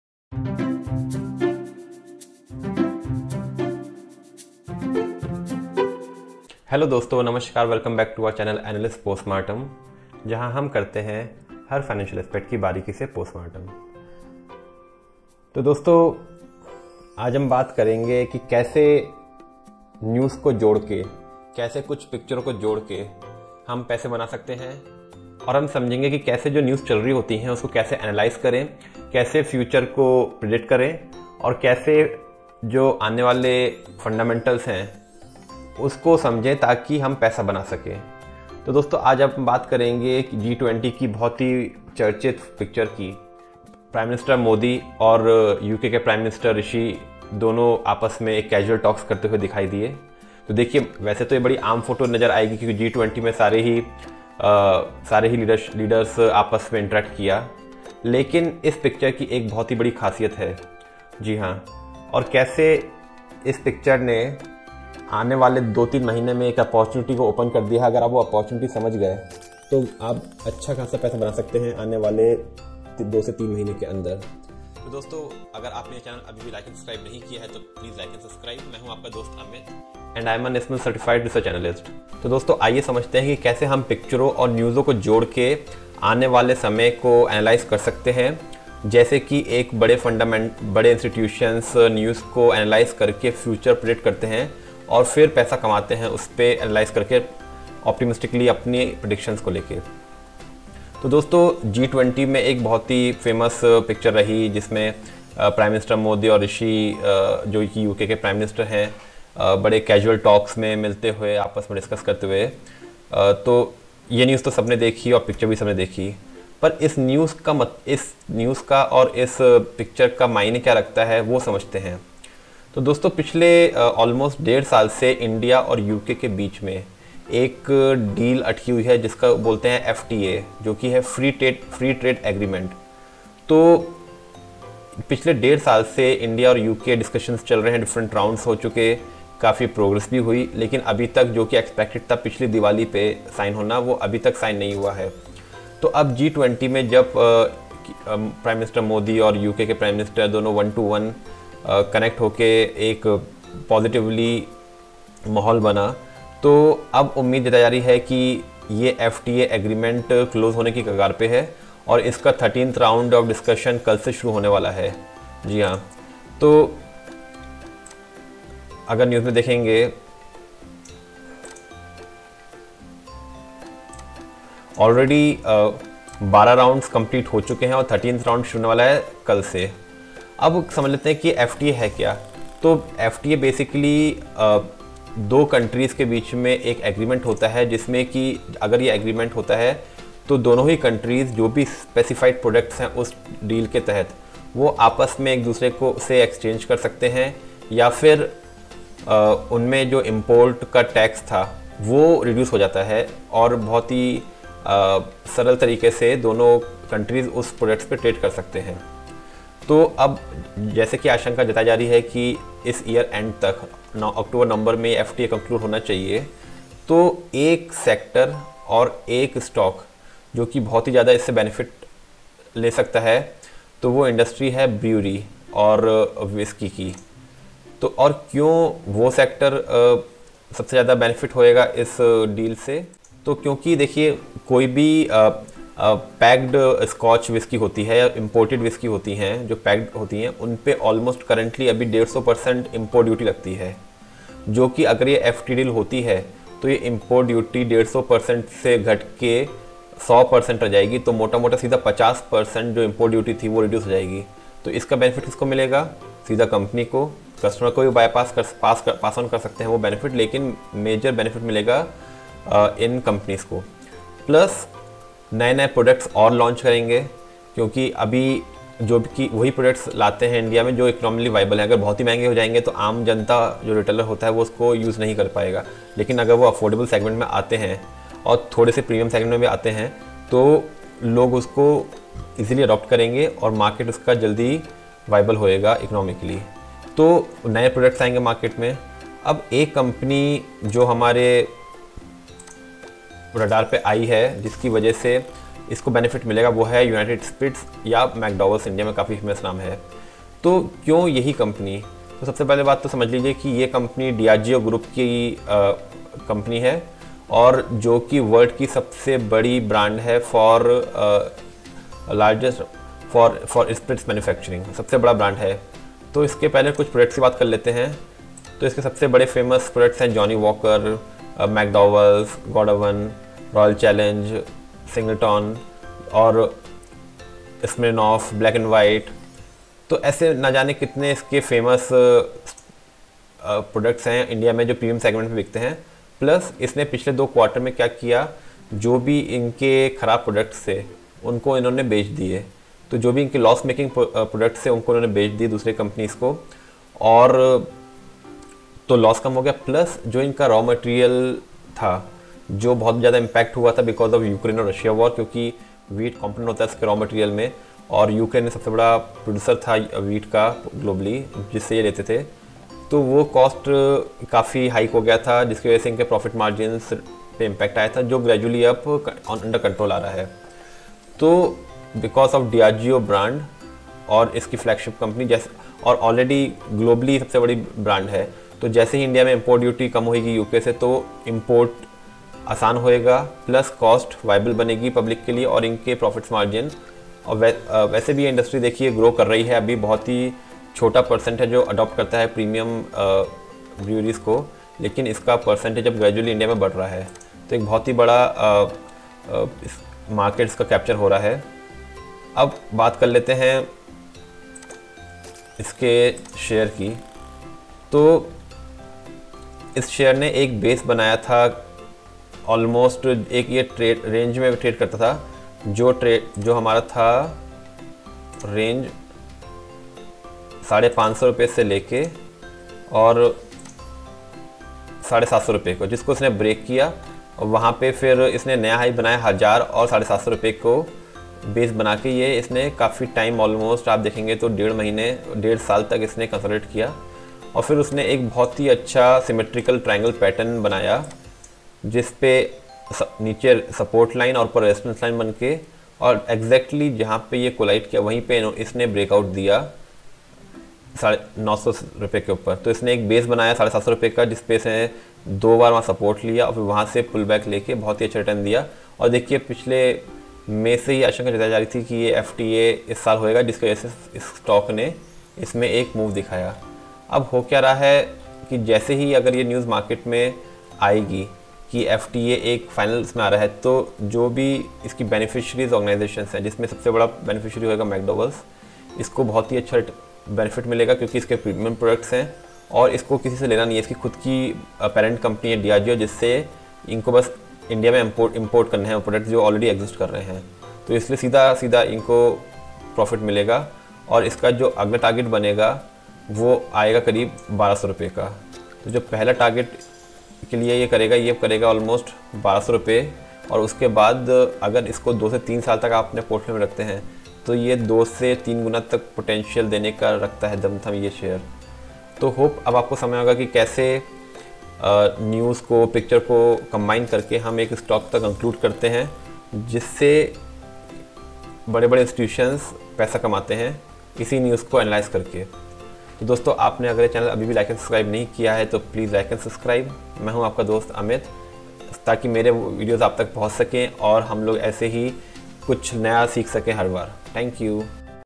McDowell stock news | After India-UK FTA Agreement mcdowells unitedspirits to 👓
About this Video:
What would an India-UK free trade deal mean for Scotch? Learn how to Analyze news, pictures in media.

dividend investing